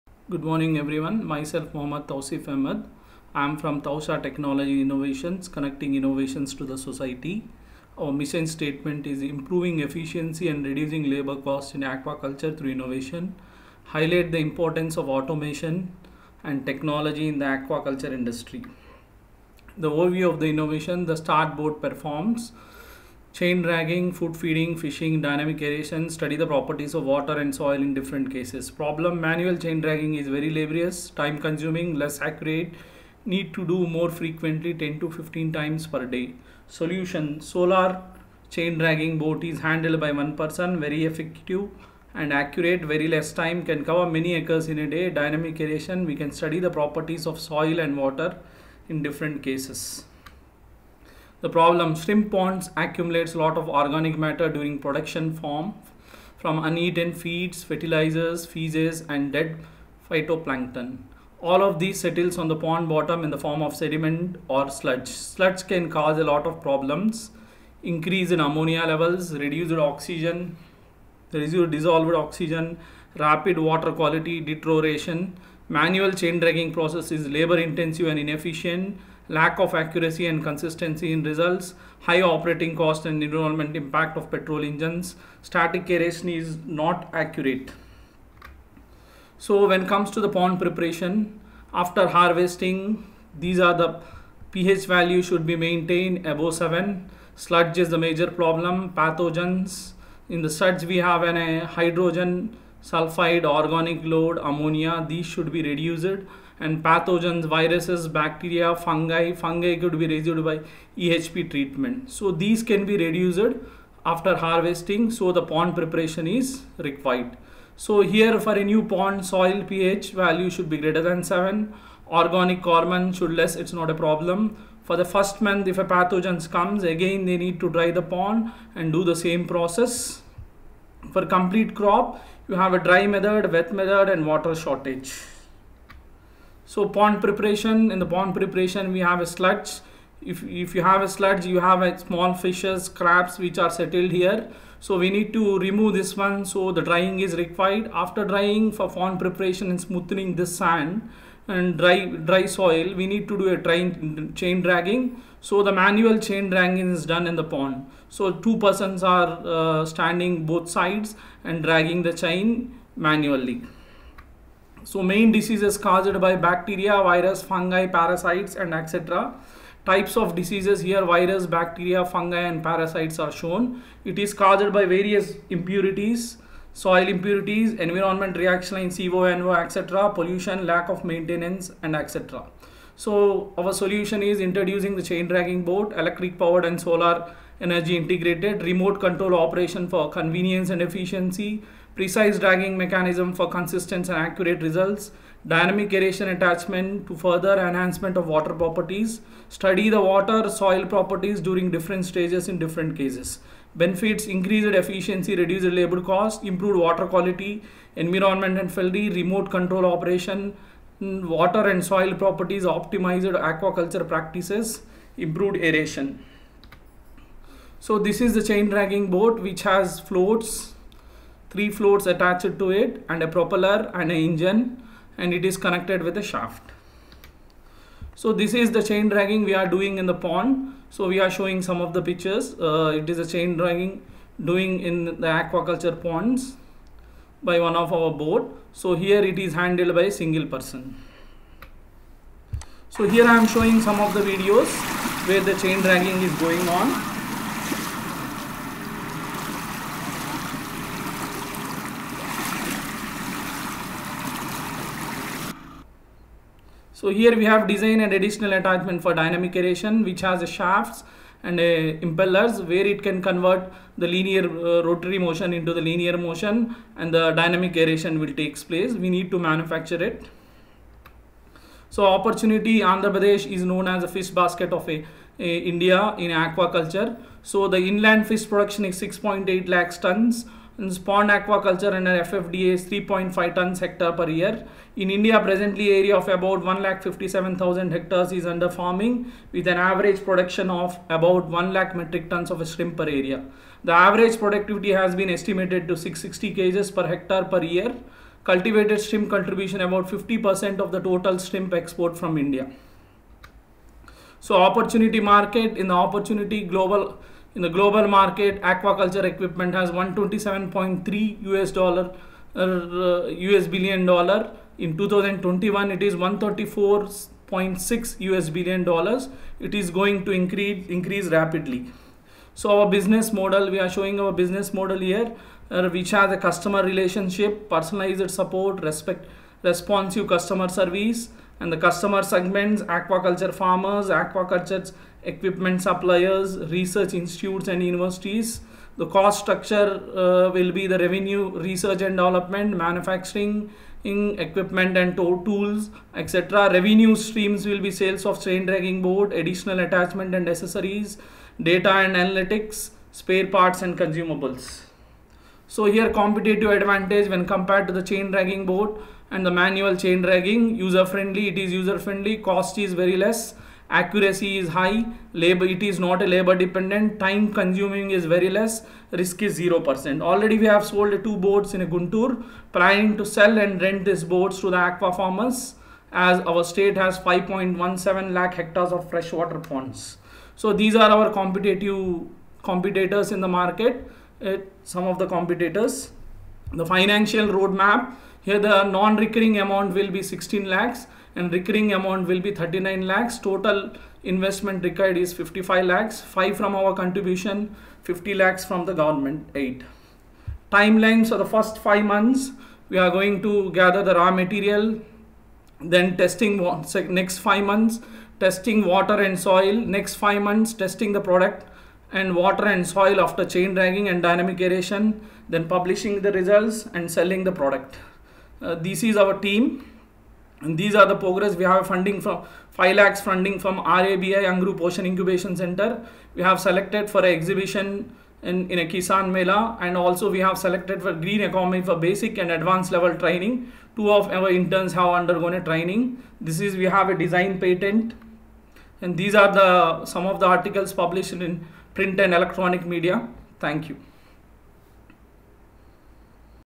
BIRACBIGPPT CHAIN DRAGGING BOAT
BIRACBIG PPT ON TECHNOLOGY ENHANCEMNT OF CHAIN DRAGGING BOAT AND ADDITIONAL ATTACHMENT FOR DYNAMIC AERATION